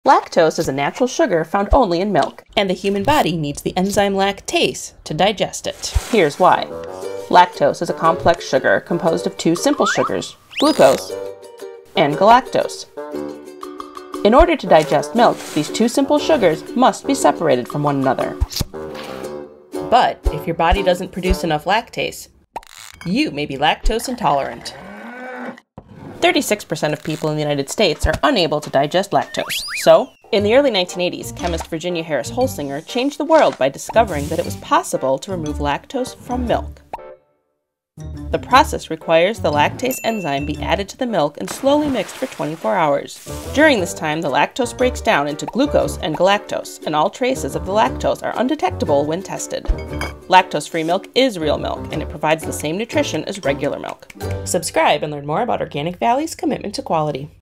What you don't know about Lactose Free milk
Organic Valley Lactose Free Milk begins as our premium, certified organic, pasture-raised milk, so what exactly is lactose and how do we remove it from our milk? Watch and learn!